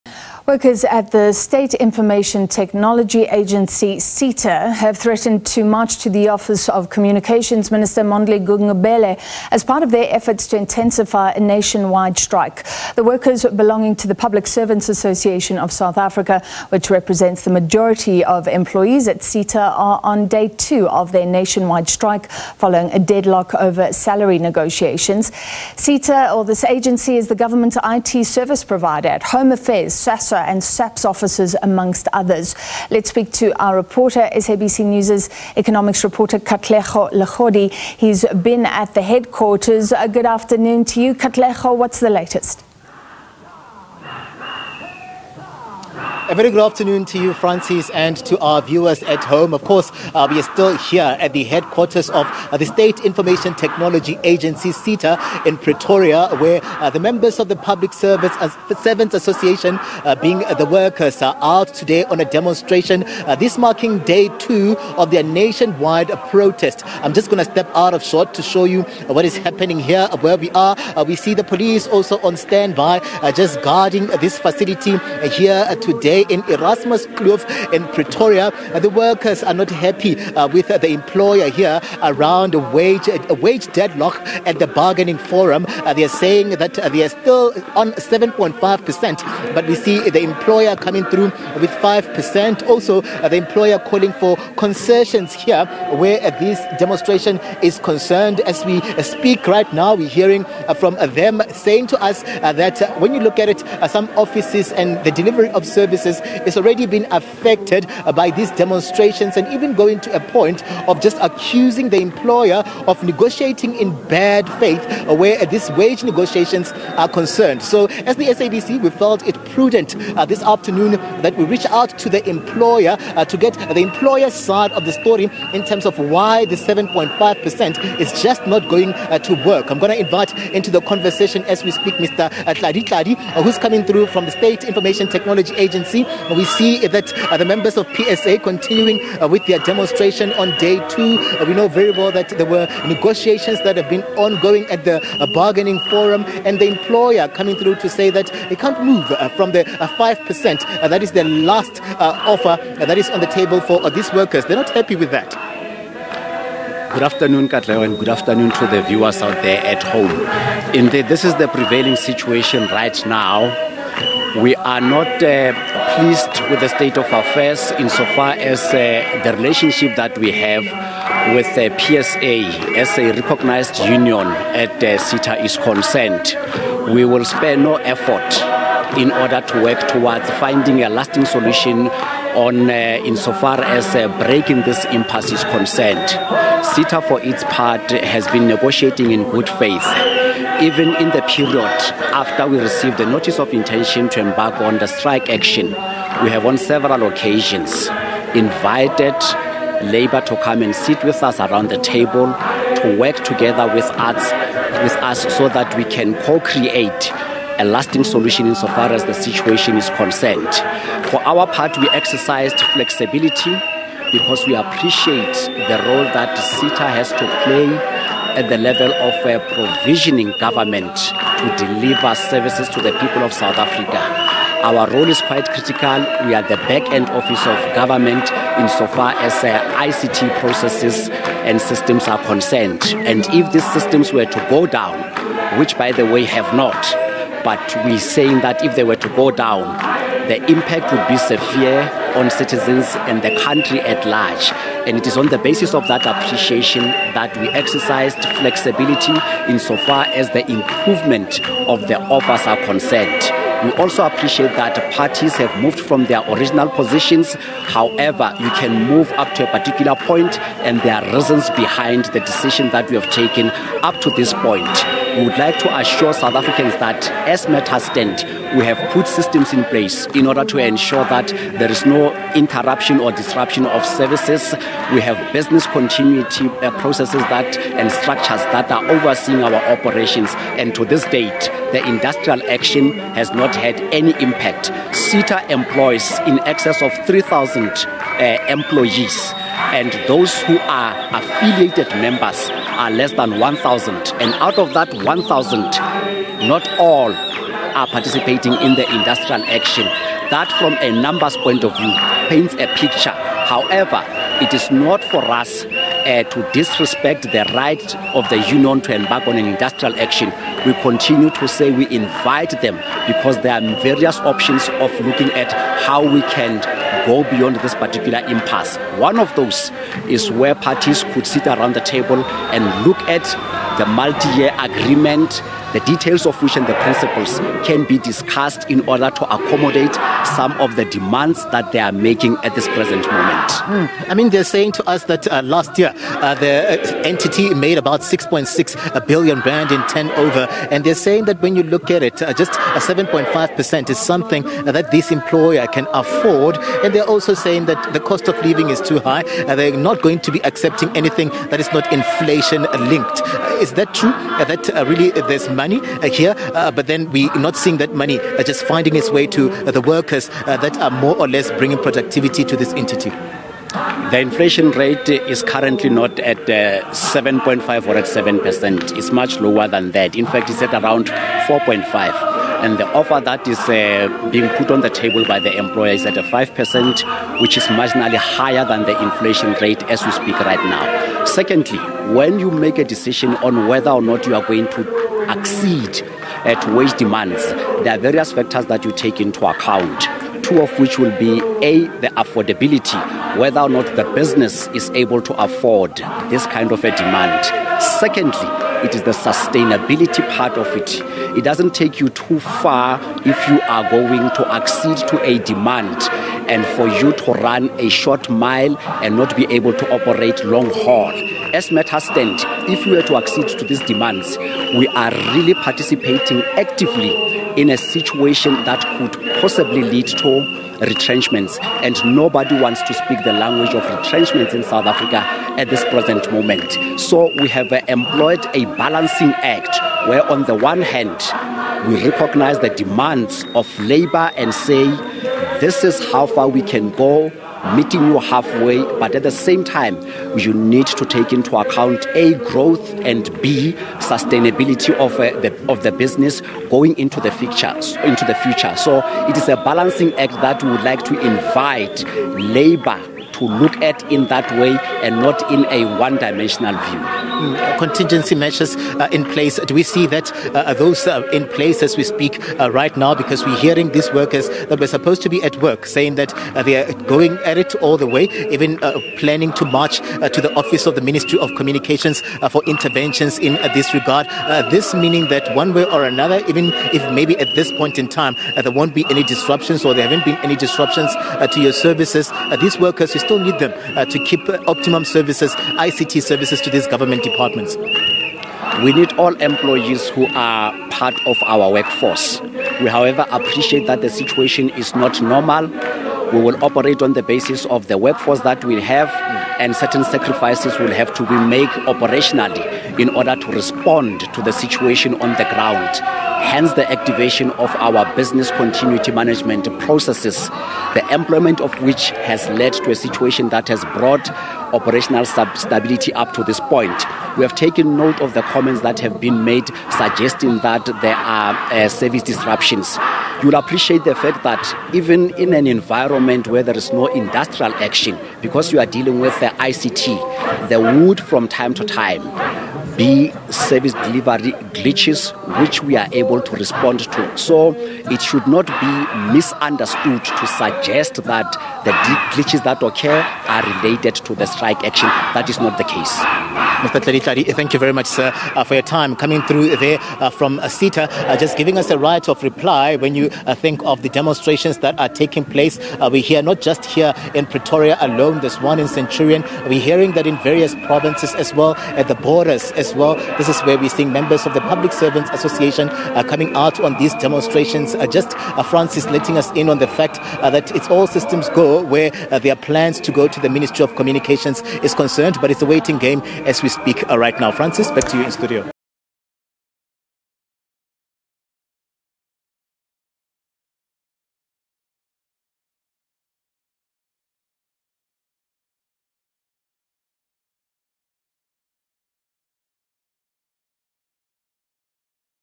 Employer insists it exercised flexibility as SITA workers vow to intensify strike: Tlali Tlali
Workers at the State Information Technology Agency have threatened to march to the office of communications minister Mondli Gungubele as part of their efforts to intensify the nationwide strike action at SITA offices.
The workers, who belong to the Public Servants Association of South Africa which represents the majority of employees at SITA, are on day 2 of their national strike following a deadlock in salary negotiations. The State Information Technology Agency is the government  IT service provider at Home Affairs, SASSA and SAPS offices amongst others.
SABC News Economics Reporter Katlego Legodi is at the SITA headquarters in Pretoria and joins us live.